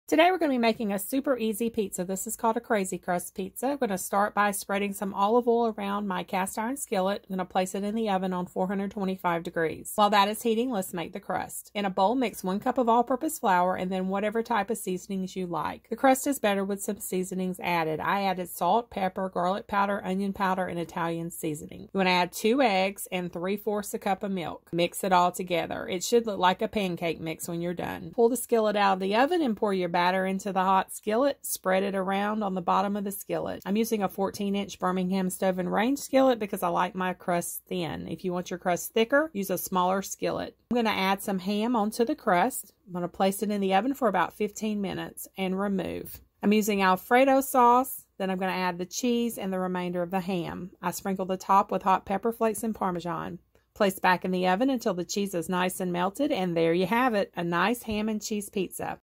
Easy Pizza - No rise crust
Today I will show you how to make a super easy crazy crust pizza. This pizza has a no rise crust. All you do is preheat oven to 425 degrees. Oil a skillet and stick in the oven to heat while preparing crust.
For the crust mix 1 cup all purpose flour, whatever seasonings you prefer, 3/4 cup milk, 2 eggs. Mix until it has a pancake mix consistency. Remove skillet from oven and pour batter in. Spread around skillet. Top with part of the ham. Place in oven and bake 15 minutes. Remove. Add your toppings. Place back in oven until cheese has melted. Enjoy!